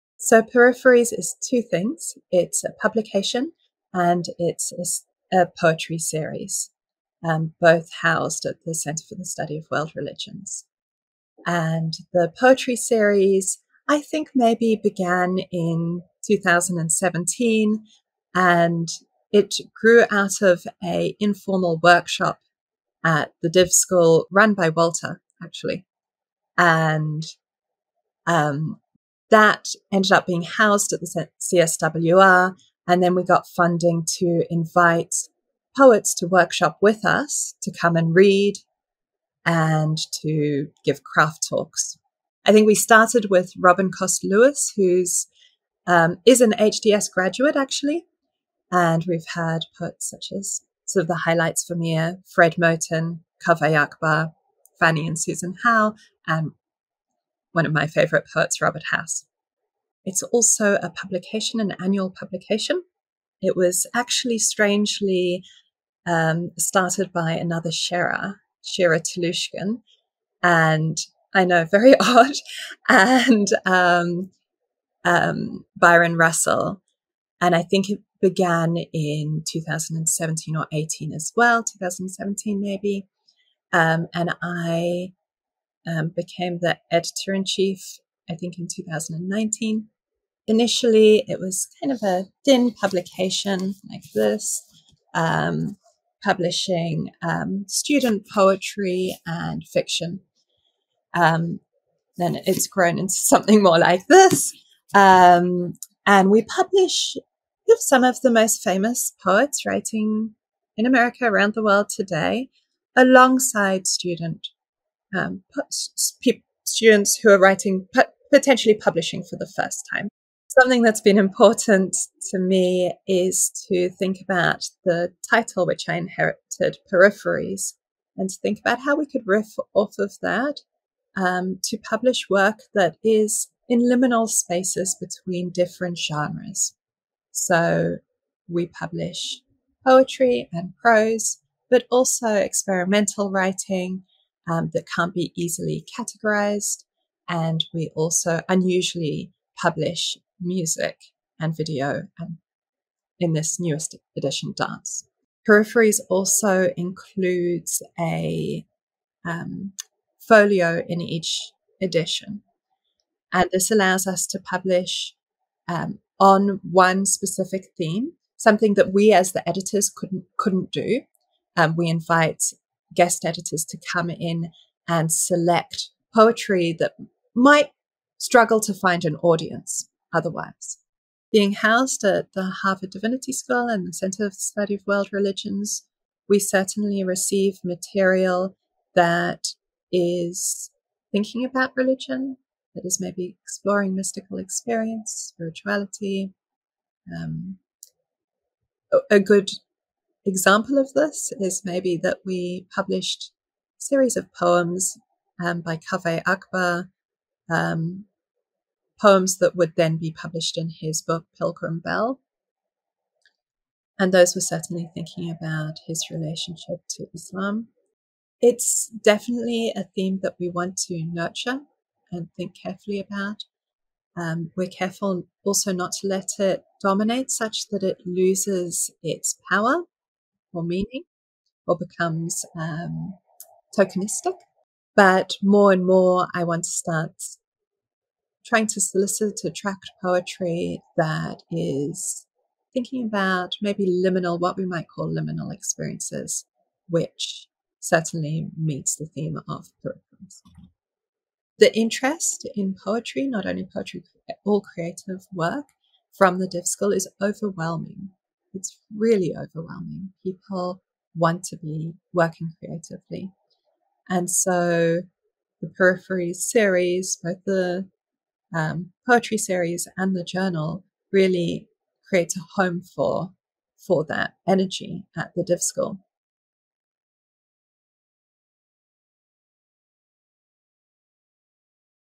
CSWR Interview: Sherah Bloor on Peripheries
Sherah Bloor is a Research Assistant at Center for the Study of World Religions at Harvard Divinity School and editor-in-chief of the literary and arts journal,  Peripheries. Here she discusses the journal and related programming.